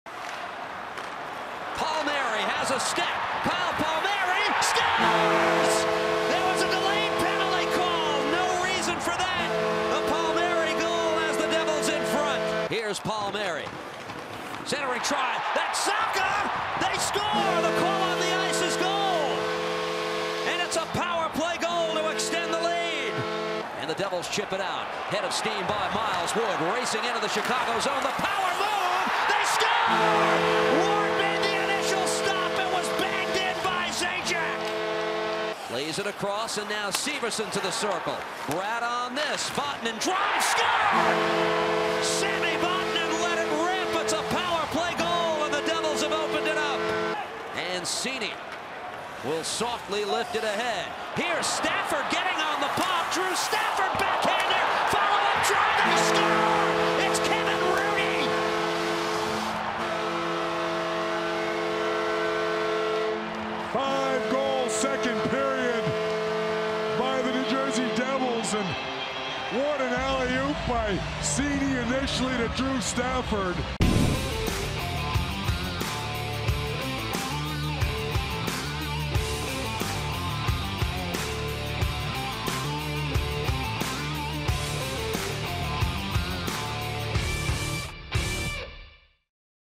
Cam Ward Smashes Stick After Giving Up Five Straight Goals To Devils
Blackhawks goalie Cam Ward was furious after giving up five consecutive goals to the Devils in the second period.